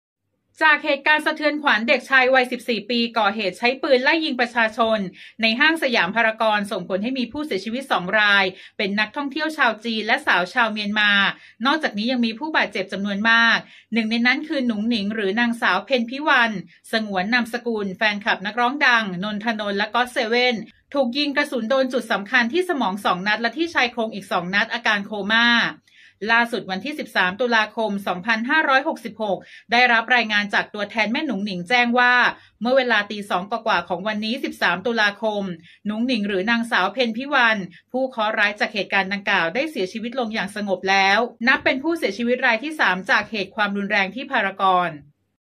เสียชีวิตอีก 1 ราย แฟนคลับ 'นนท์ ธนนท์-GOT7' เหยื่อกราดยิงยิงพารากอน
จากเหตุการณ์สะเทือนขวัญ เด็กชาย วัย 14 ปี ก่อเหตุใช้ปืนไล่ยิงประชาชน ในห้างสยามพารากอน ส่งผลให้มีผู้เสียชีวิต 2 ราย เป็น นักท่องเที่ยวจีน และ สาวชาวเมียนมา นอกจากนี้ยังมีผู้บาดเจ็บจำนวนมาก หนึ่งในนั้นรวมถึง หนุงหนิง น.ส.เพ็ญพิวรรณ (สงวนนามสกุล) แฟนคลับนักร้องดัง นนท์ ธนนท์ และ GOT7 ถูกยิงกระสุนโดนจุดสำคัญที่สมอง 2 นัด และที่ชายโครงอีก 2 นัด อาการโคม่า

โดยหลังเกิดเหตุ เมื่อวันที่ 3 ต.ค.ที่ผ่านมา นนท์ ธนนท์ รีโพสต์ข้อความจากแฟนคลับ ตามหาคนหายจากเหตุการณ์ไล่ยิงคนในห้าง พารากอน ก่อนจะพบว่า เจอหญิงสาวคนดังกล่าวแล้ว โดนยิงที่ศีรษะ อยู่โรงพยาบาล นั้น

ล่าสุดวันที่ 13 ต.ค.2566 ผู้สื่อข่าวได้รับรายงาน จากตัวแทนแม่หนุงหนิงแจ้งว่า เมื่อเวลาตี 2 กว่าๆ ของวันนี้ (13 ต.ค.) หนุงหนิง หรือ น.ส.เพ็ญพิวรรณ ผู้เคราะห์ร้ายจากเหตุการณ์ดังกล่าว ได้เสียชีวิตลงอย่างสงบแล้ว นับเป็นผู้เสียชีวิตรายที่ 3 จากเหตุความรุนแรงที่พารากอน